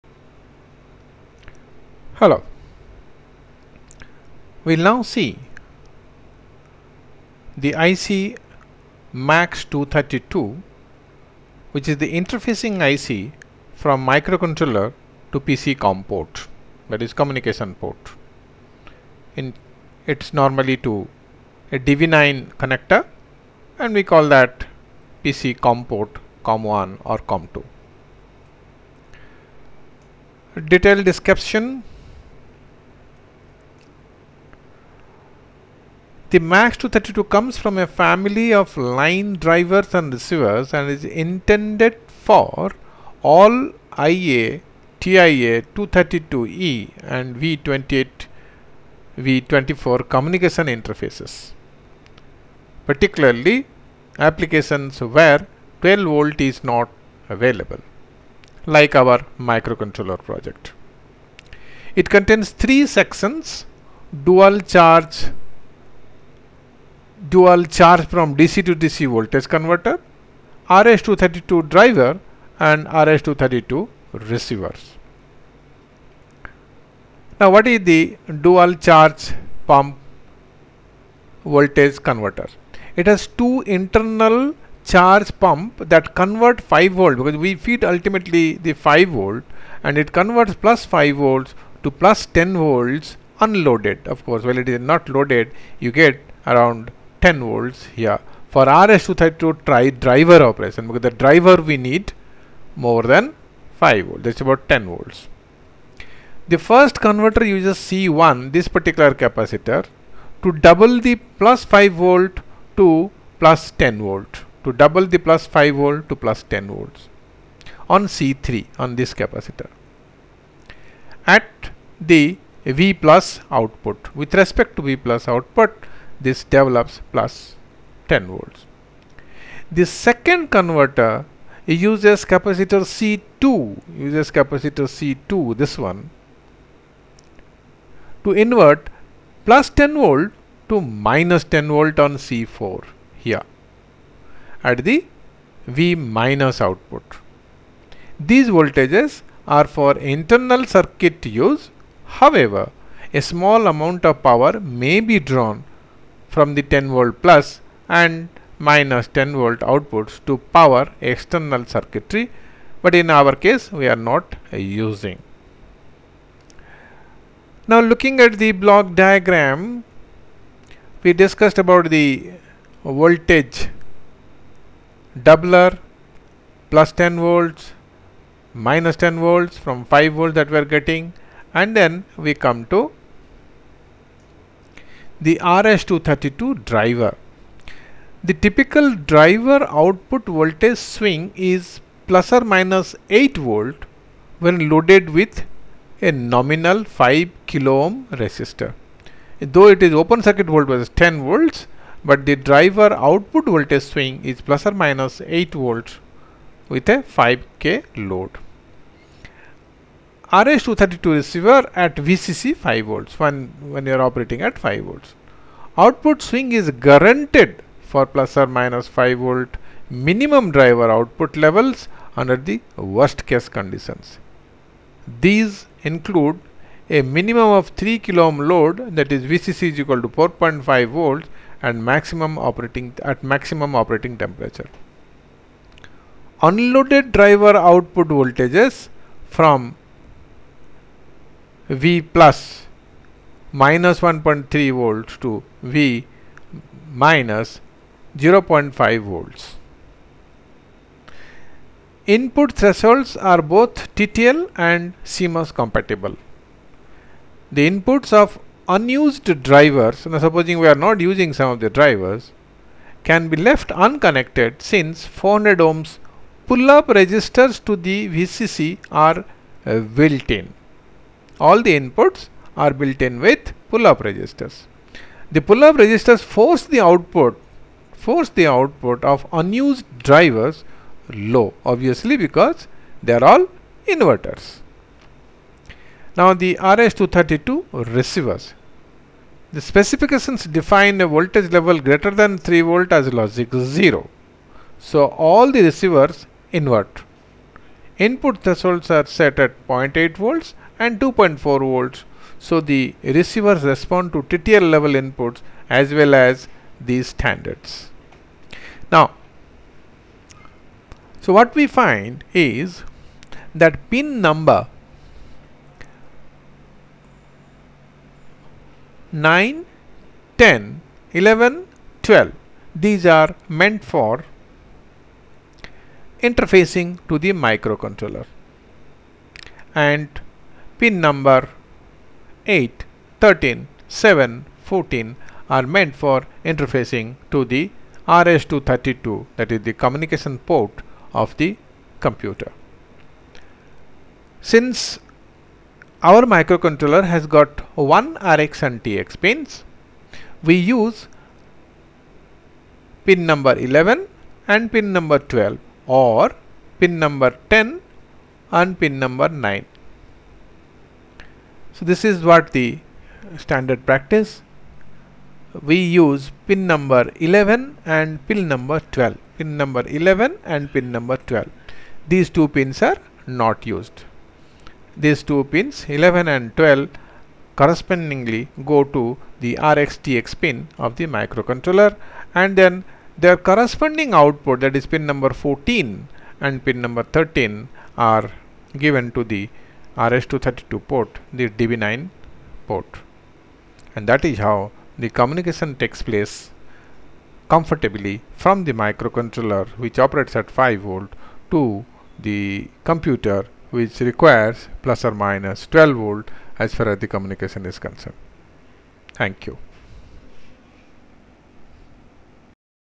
Max232, The level shifter IC for microcontroller interfece.avi
The level shifter IC that makes interface from 5 Volt DC signal to 12 volt.For example from microcontroller to a computer com port